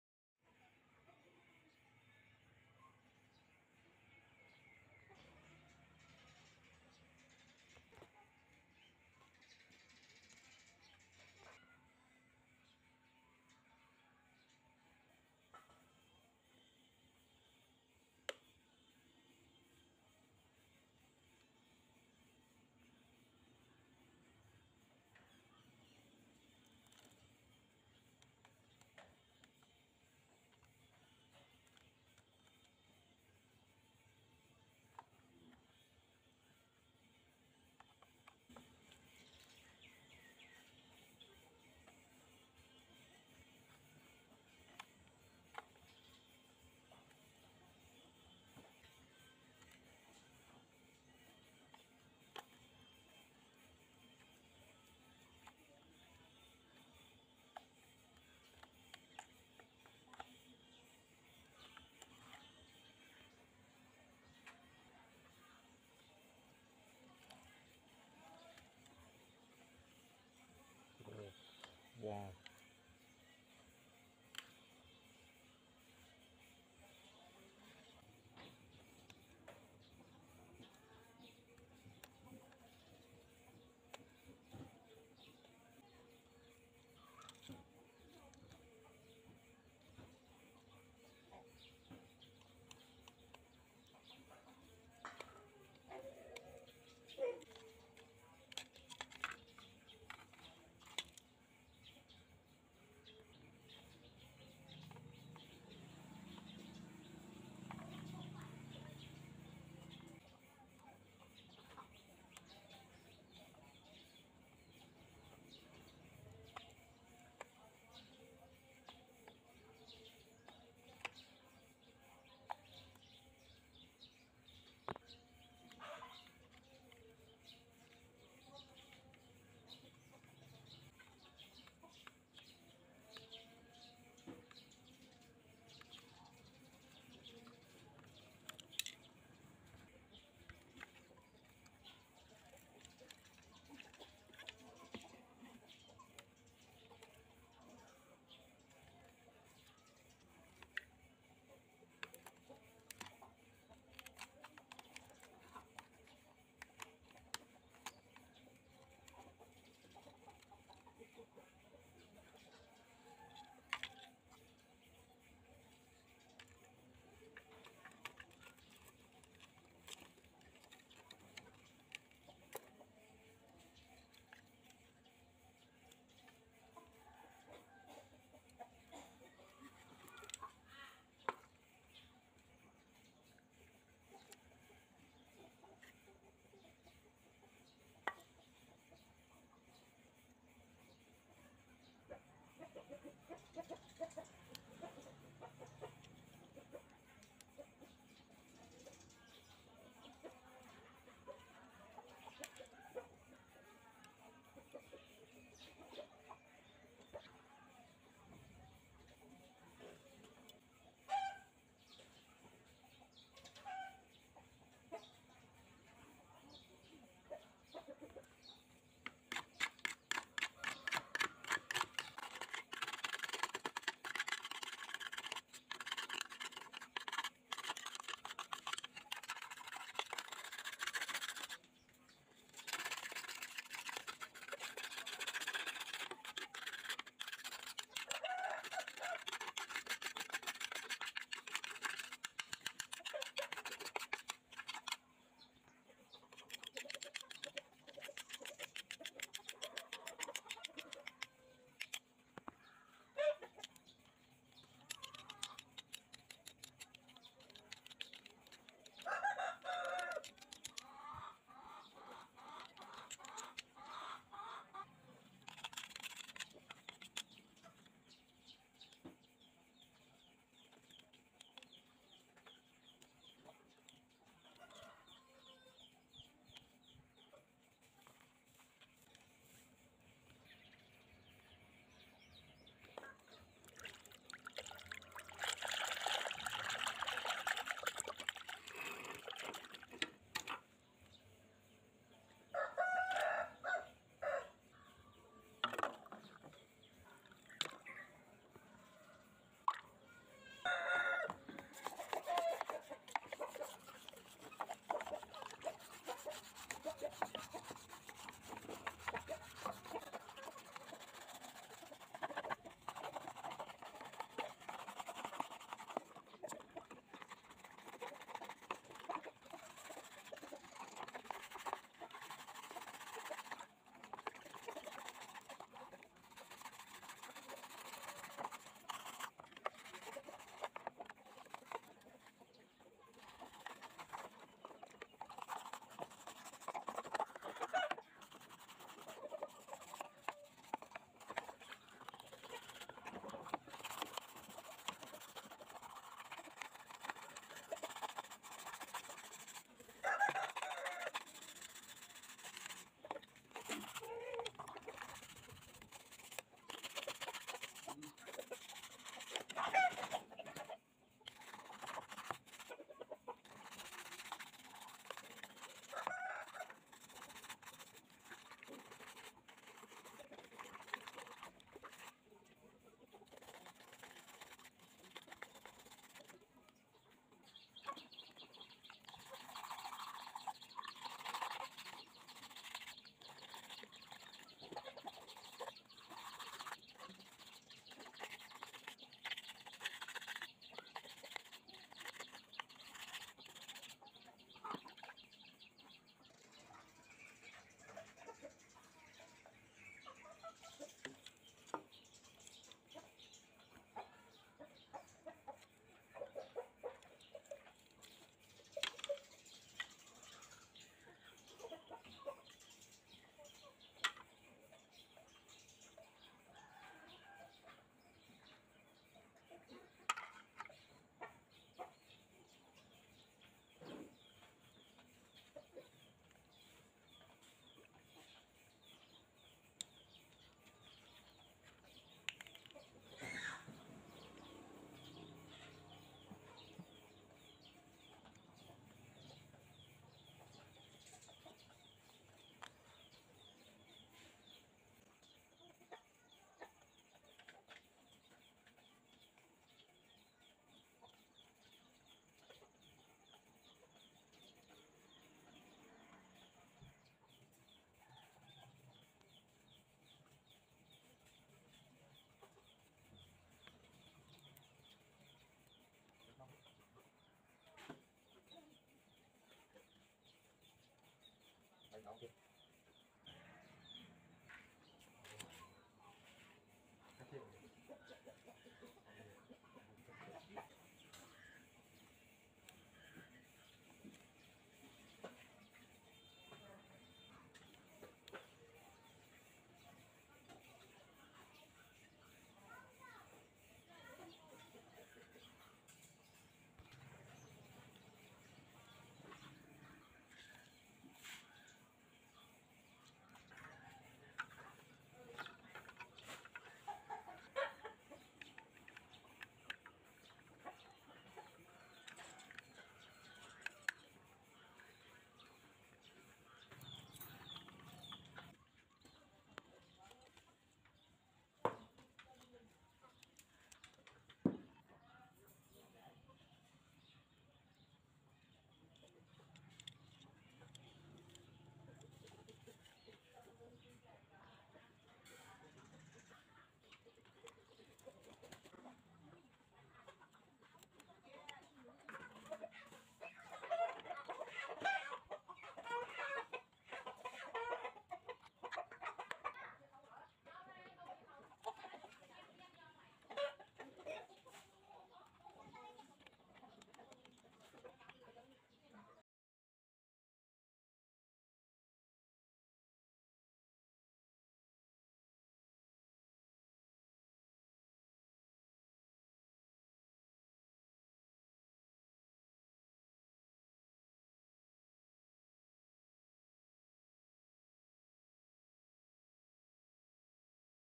Old rusty F919 radio aircraft 30 years old, amazing restoration
Old rusty F919 radio aircraft
#30 years old,
# amazing restoration